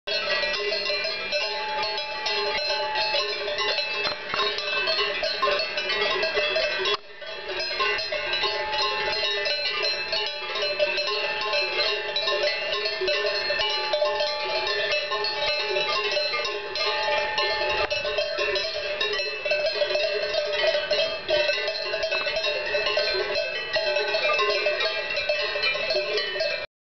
cows
Cow in field in Morschach Switzerland.